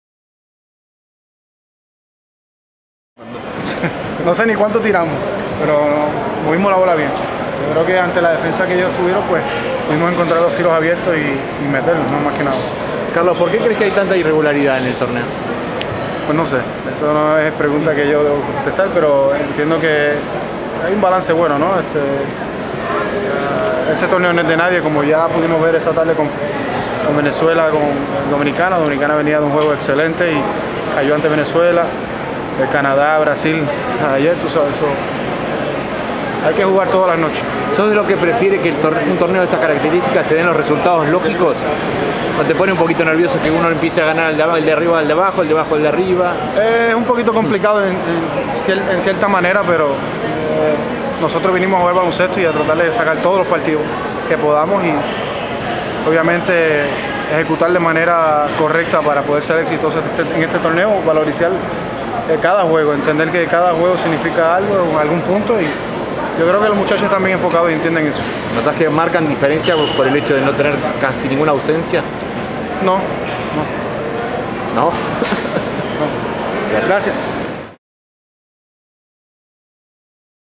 Carlos Arroyo: "Este torneo no es de nadie"
El puertorriqueño dice que ellos no tienen ventajas por no tener ausencias y que el campeonato será muy parejo.